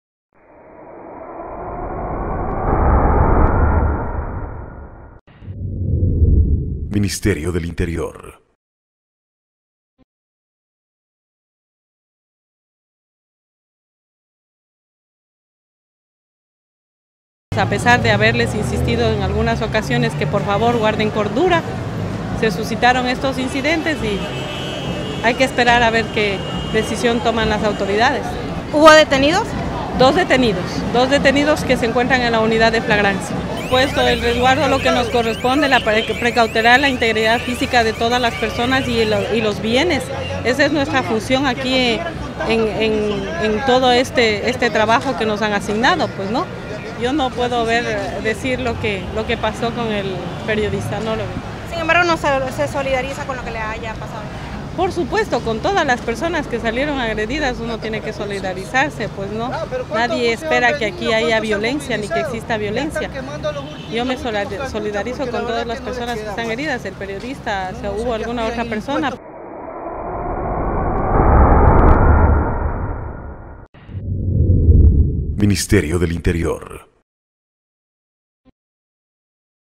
Policía Nacional evitó confrontaciones entre concentraciones a favor y en contra del gobierno
Más de mil efectivos policiales se desplegaron este jueves 7 de abril, en el operativo preventivo disuasivo y de control público, con el fin de garantizar el orden y la seguridad ciudadana, durante la marcha de la oposición  que rechaza las reformas tributarias enviadas con el carácter de económico urgente al congreso nacional.